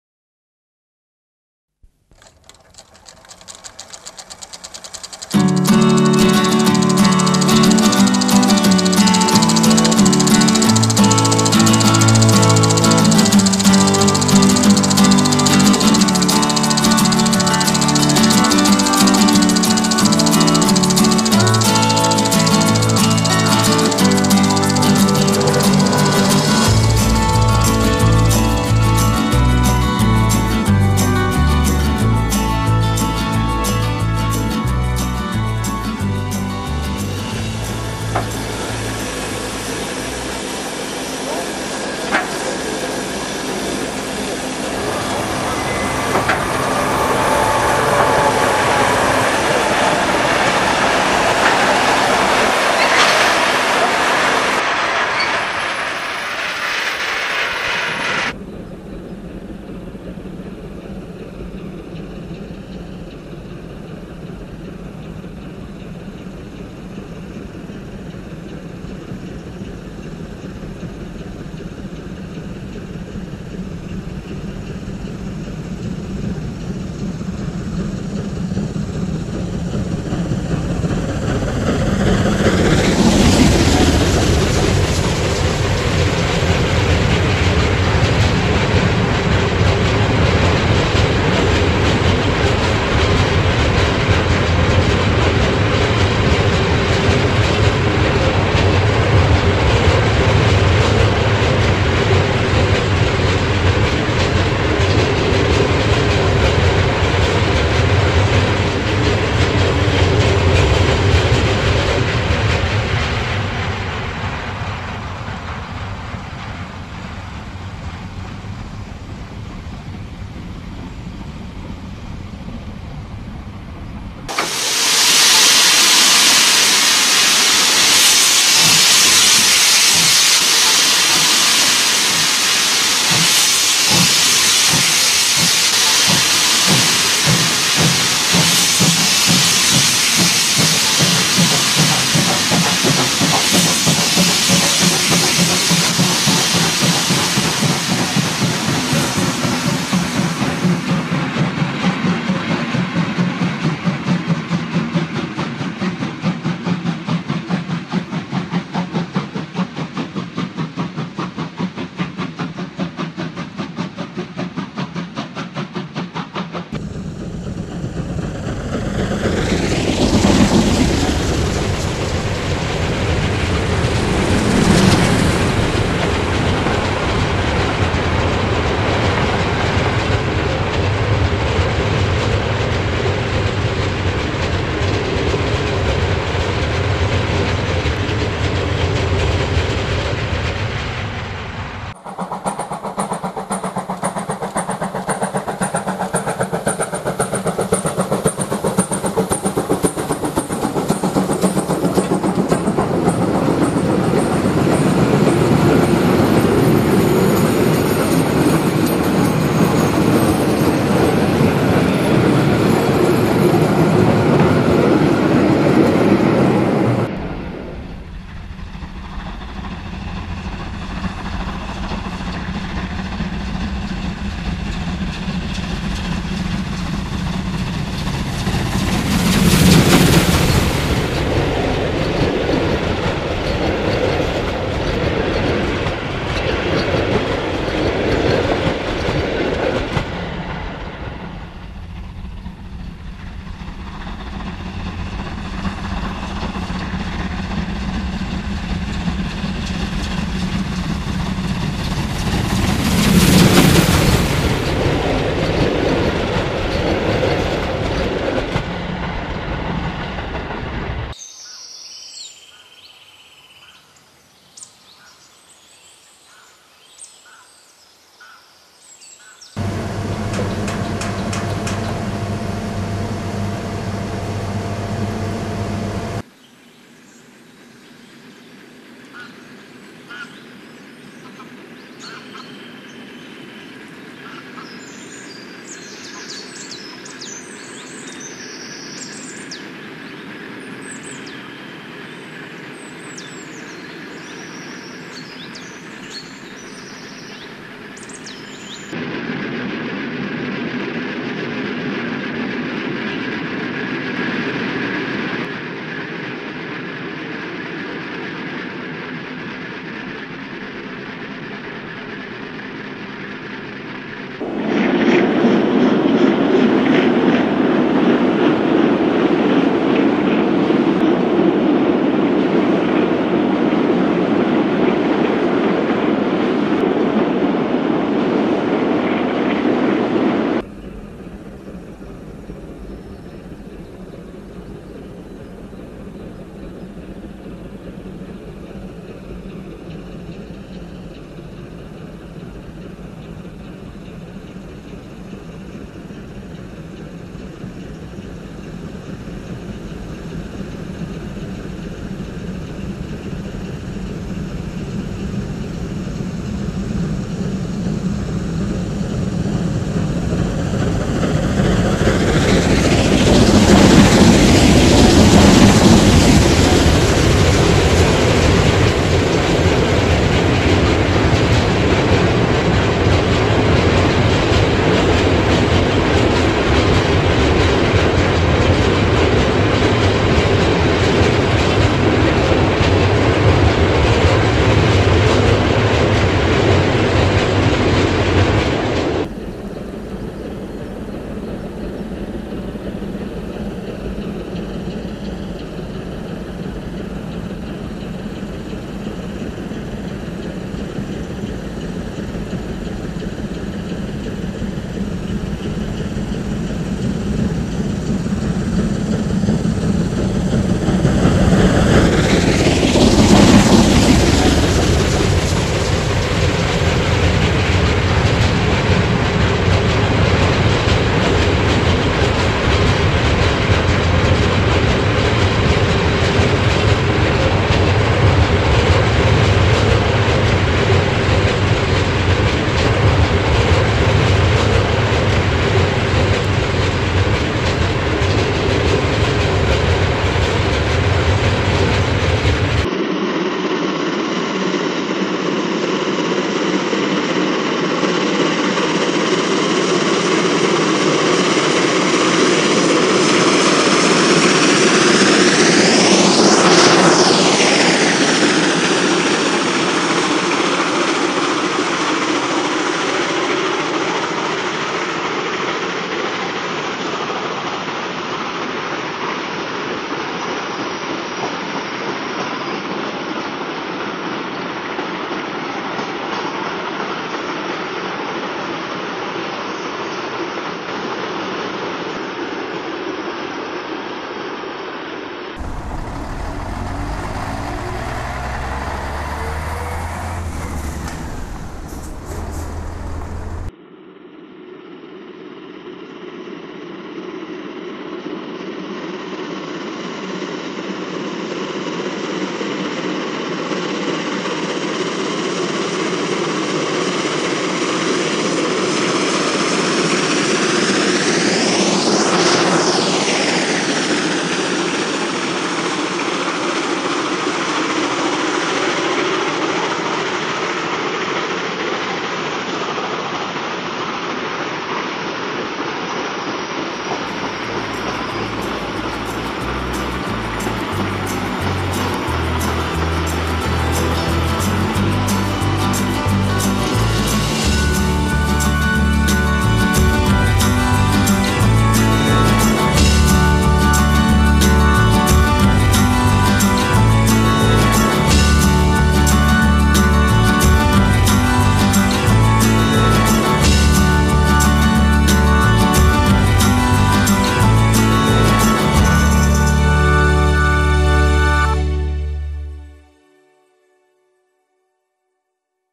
Plandampf im Emsland 1975 Teil 1
Im Februar 1975 besuchten wir zu dritt das Emsland, um den Dampfbetrieb der DB in dieser Ecke von Deutschland kennenzulernen.